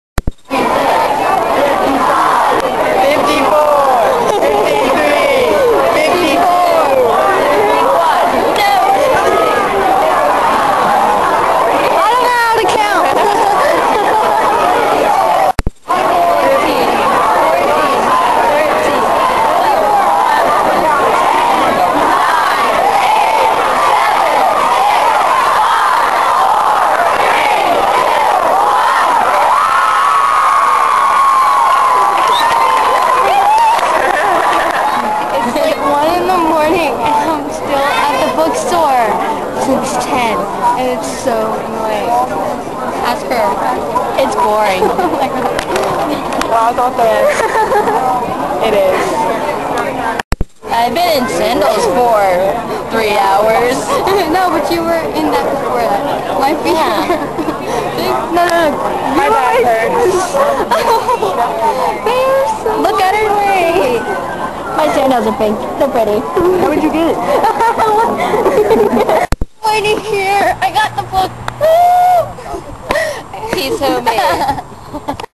Harry Potter Book 7 Blog!
My blog at 12 midnight waiting for the book!